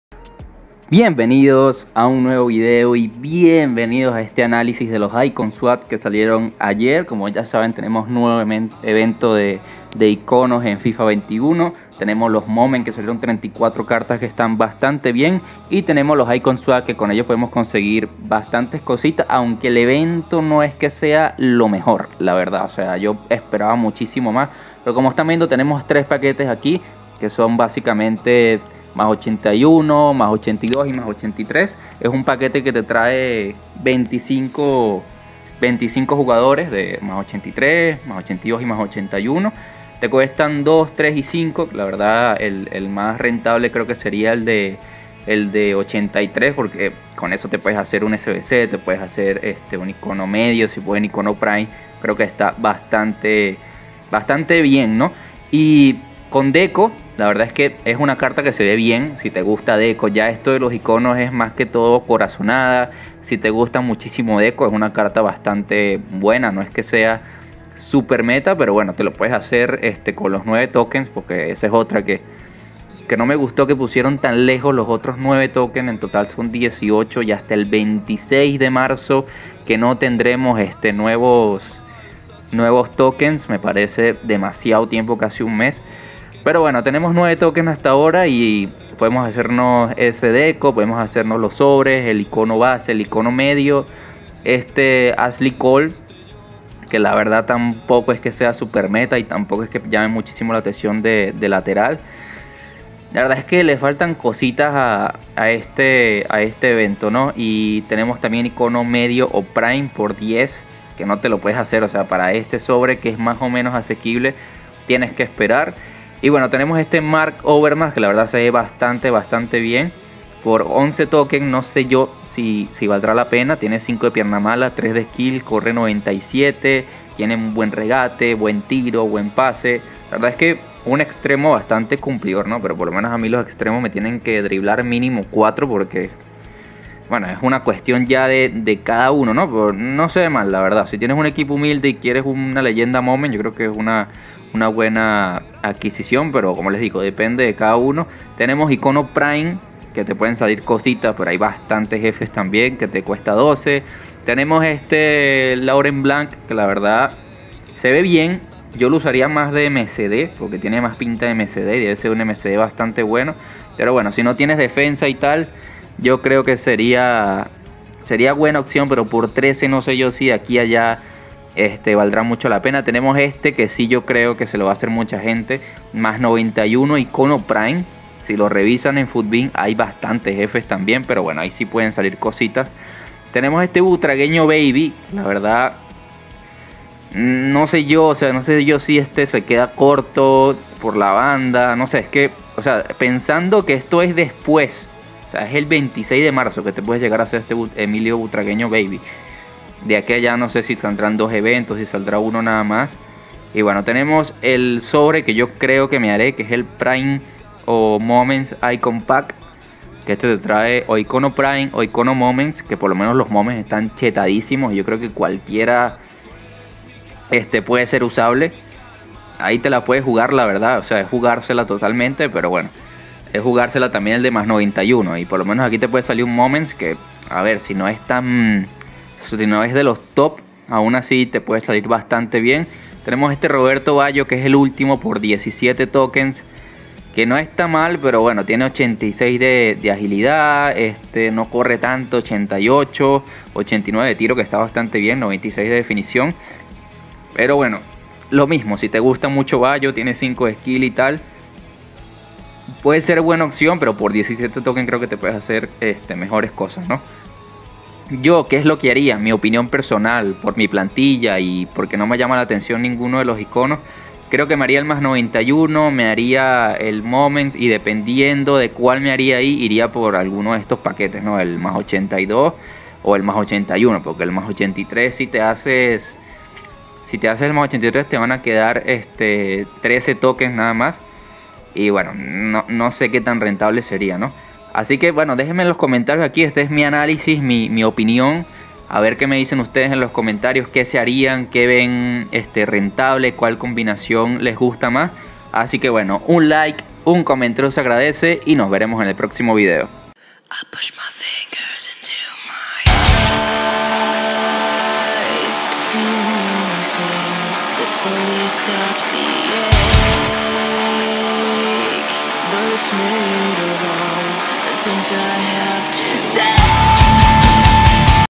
ICON SWAPS 2!! CUAL ELEGIR? SOBRES O ICONOS MOMENTS? LA MEJOR OPCION?
Hoy tenemos un analisis de las opciones de los icons swaps 2! espero les guste compartan su opinion en los comentarios!
un like y un comentario se agradece :D